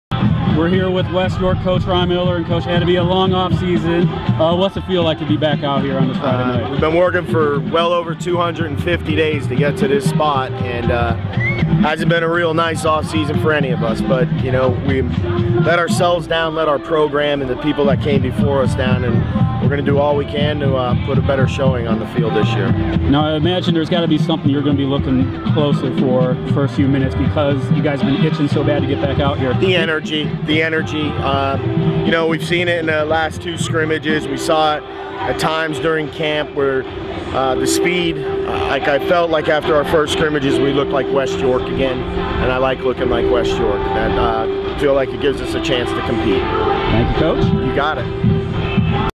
After a 4-6 season, I asked West York coach Ron Miller how badly his players wanted to get back on t
After a 4-6 season, I asked West York coach Ron Miller how badly his players wanted to get back on the field.

By: Matt Goul - York Daily Record/Sunday News and GameTimePA.com reporter. Check us out at GameTimePA.com.